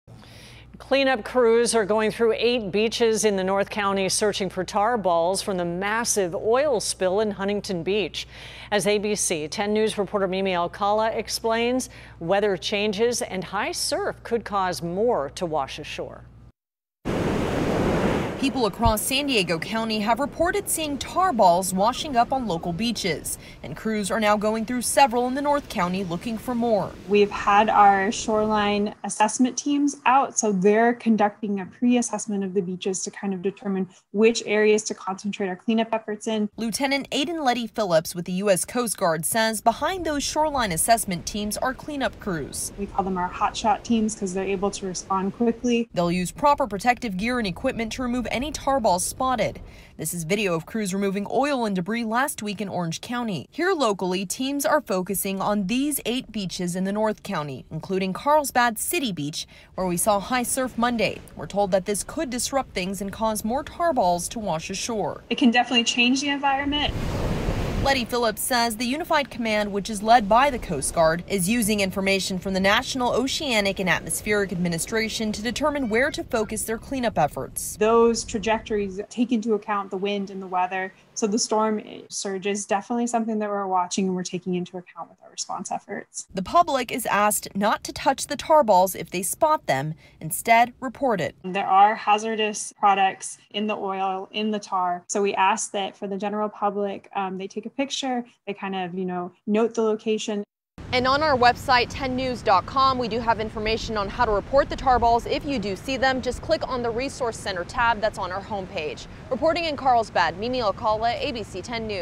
Crews search for tar balls in the North County
The U.S. Coast Guard asks beachgoers in San Diego County to be aware of more tar balls possibly washing up local beaches due to weather changes and high surf.